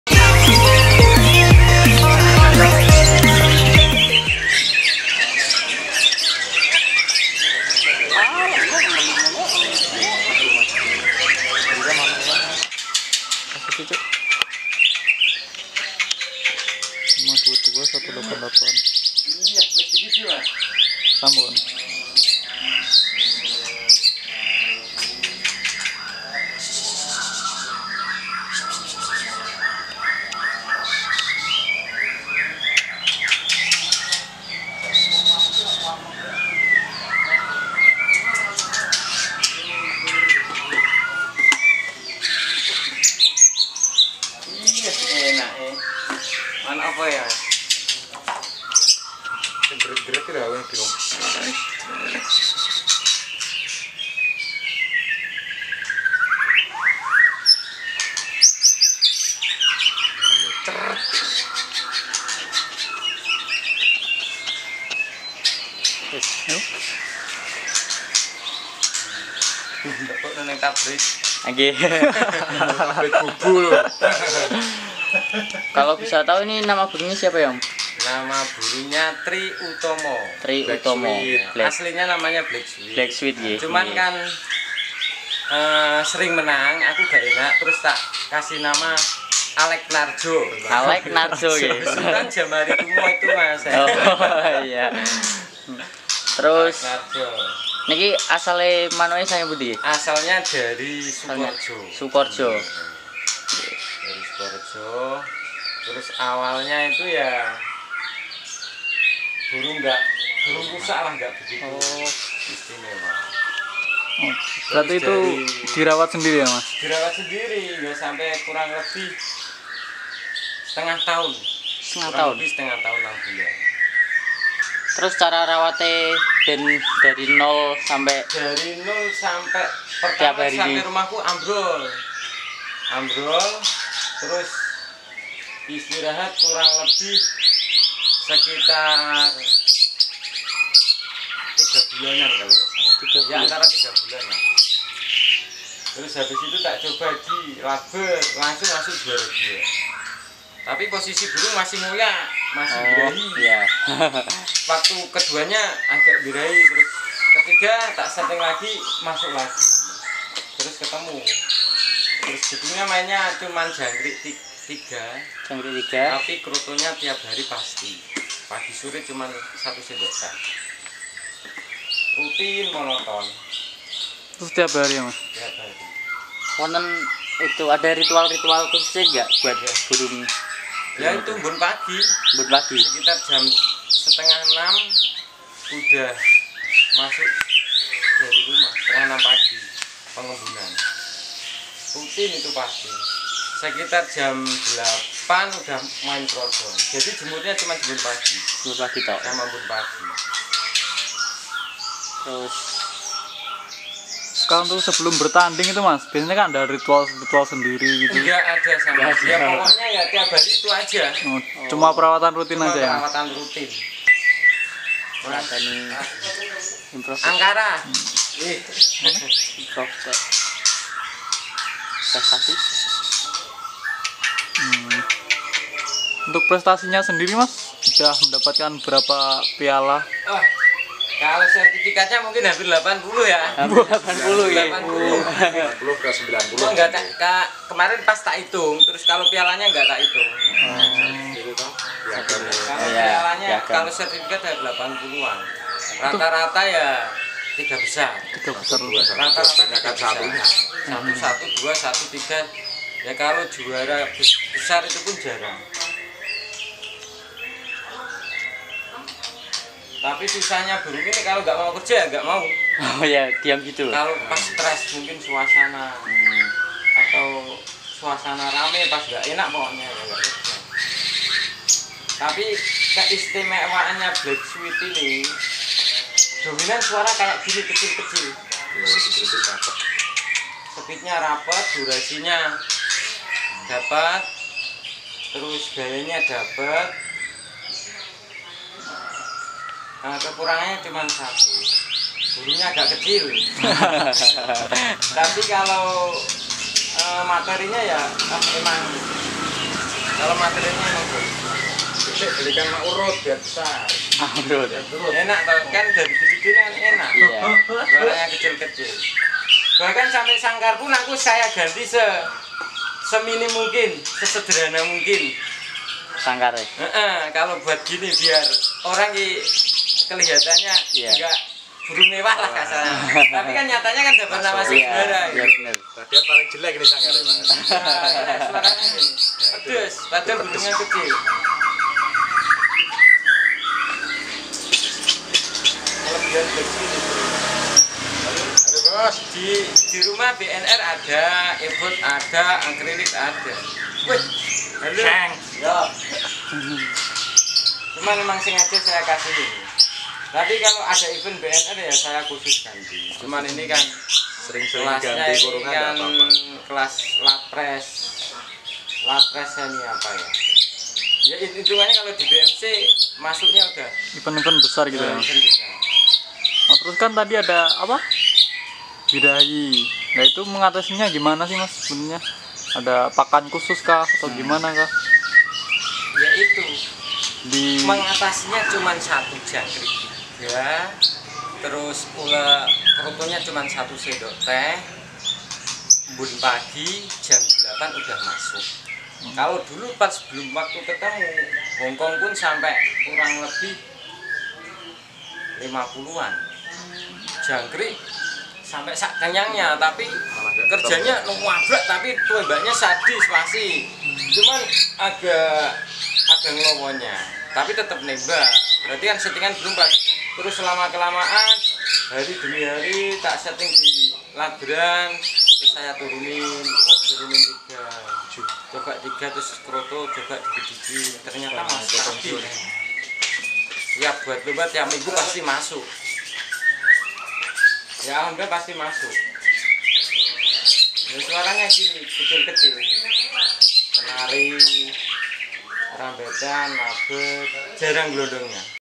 Perawatan murai batu juara blacksweet bang jack
Murai batu bang jack murai batu fighter yang sering juara di event cup bahkan kelas nasional ,apasih rahasia perawatannya sampai bisa juara.
Jangan lupa agar murai batu anda gacor berikan suplemen sup plus bird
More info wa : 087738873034